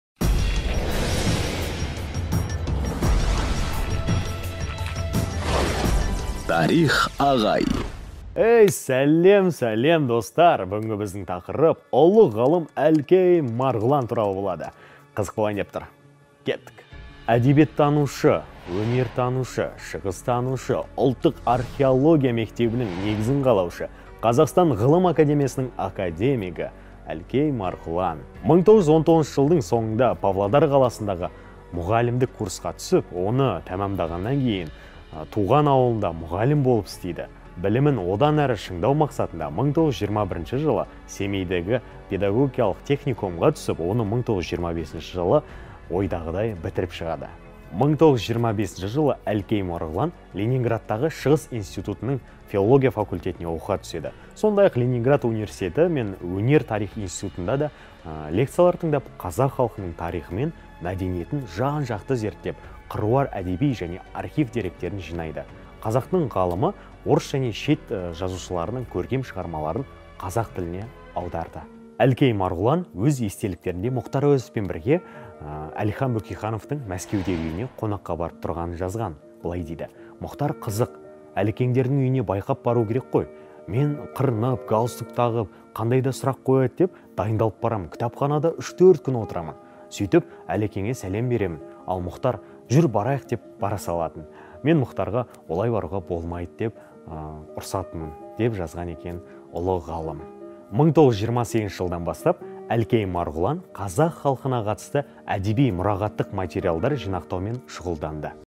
Тарихағай: Әлкей Марғұлан
00:00 - Басы
00:17 - Әлкей Марғұлан кім?
01:15 - Марғұлан естеліктері
01:52 - Әлкейдің зерттеулері